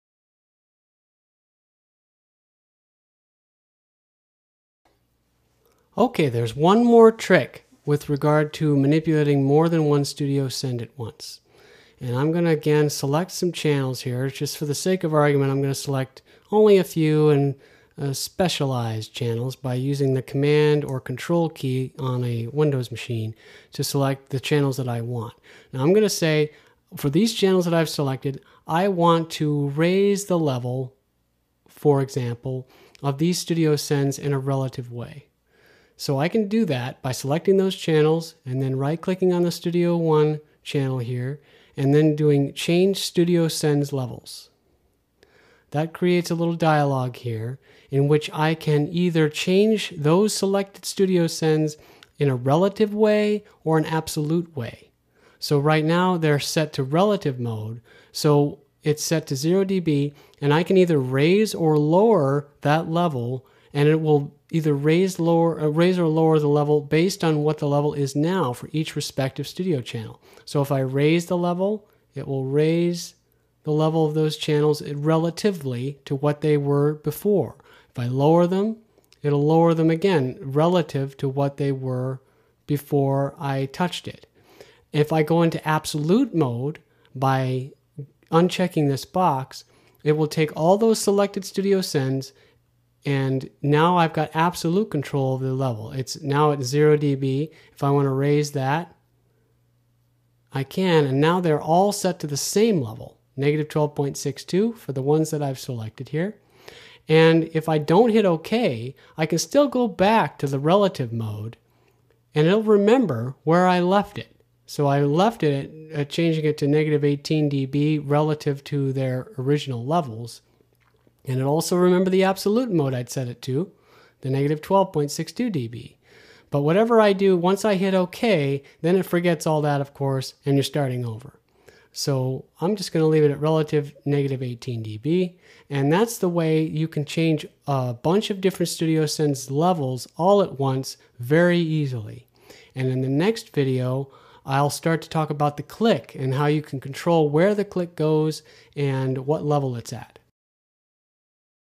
Demystifying the Control Room In Cubase - Part 9 - Studio Sends 4
Part 9 of 18 Tutorial Videos that demonstrate the features of Cubase's Control Room in a step by step and clear way.  In this video, we'll finish out exploration of studio sends.